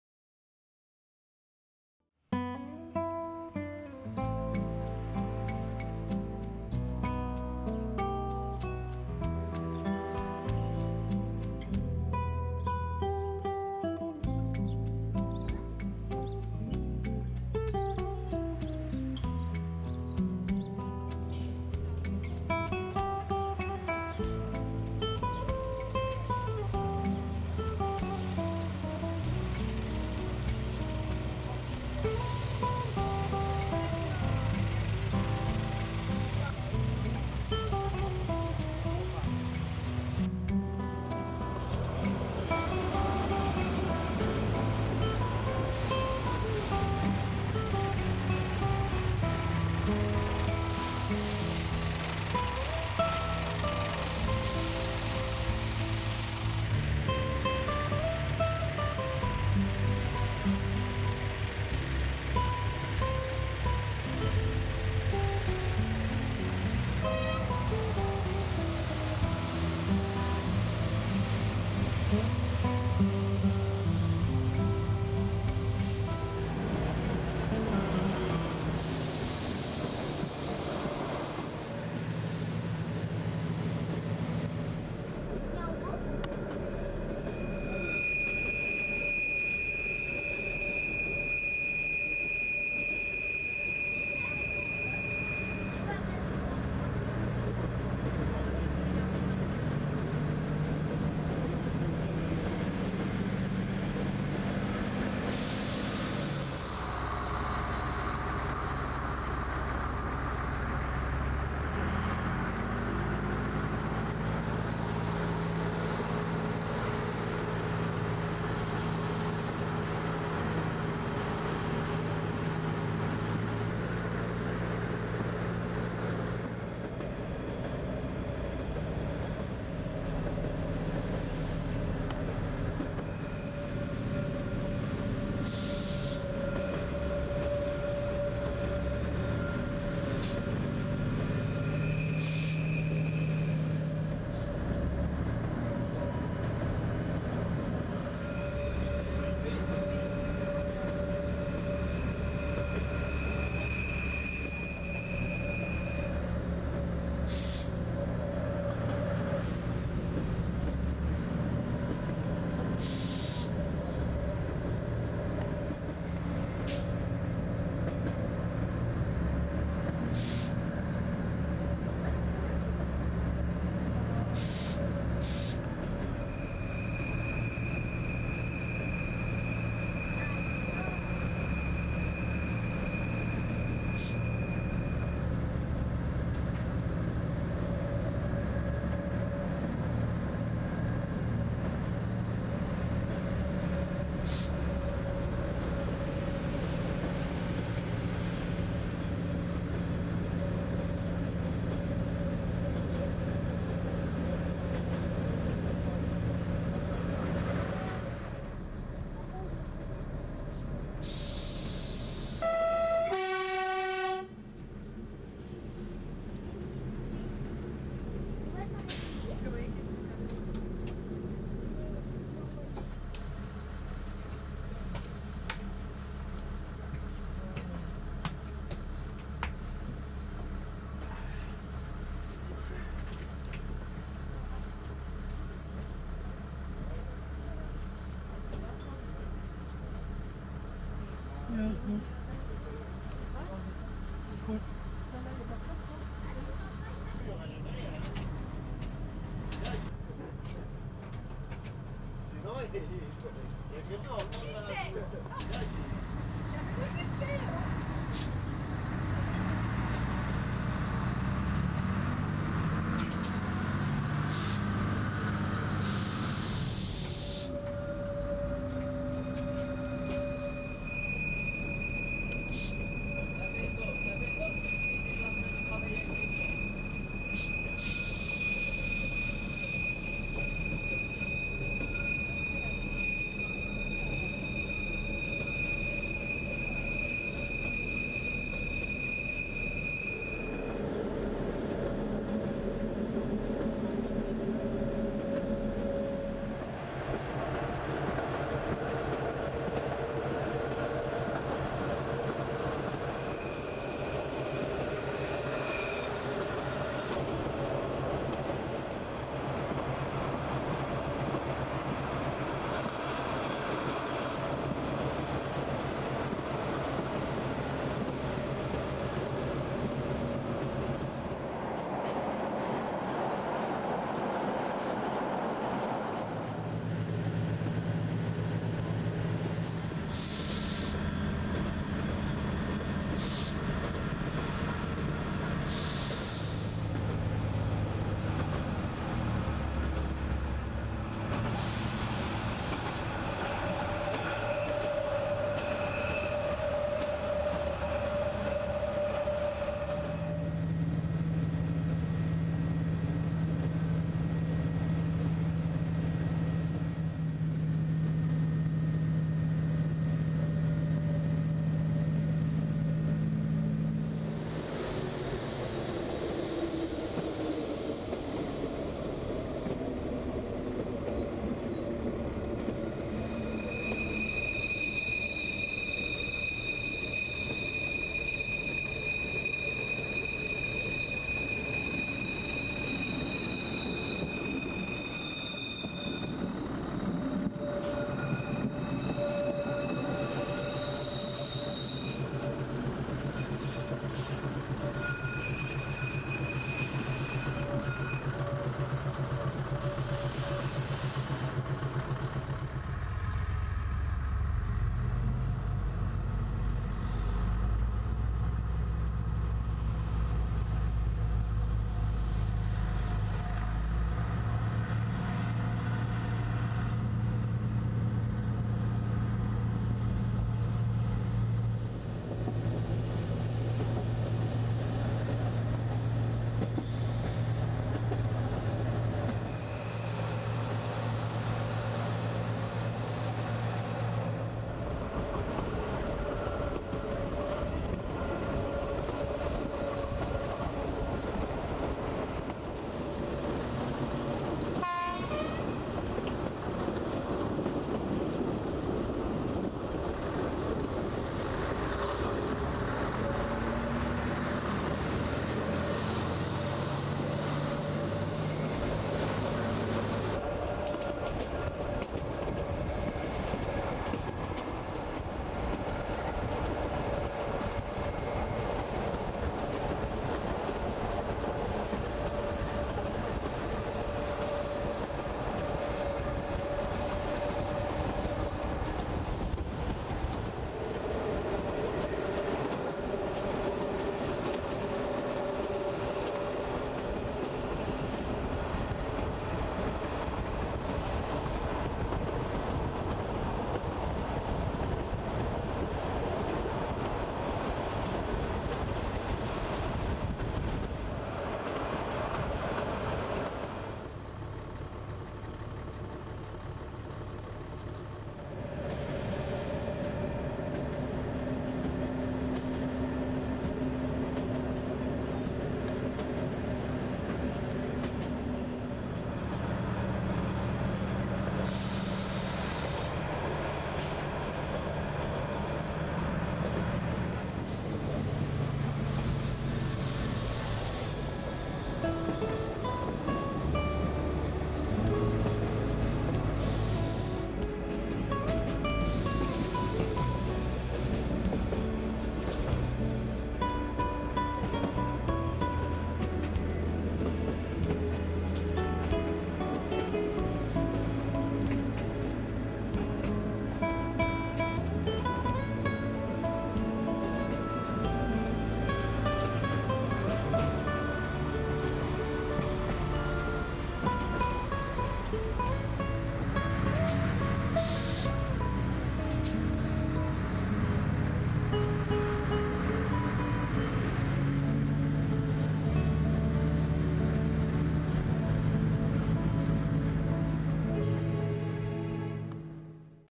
MET DE TREIN DOOR CORSICA
Met de trein dwars over het eiland en door de bergen.  [FR10-COR02]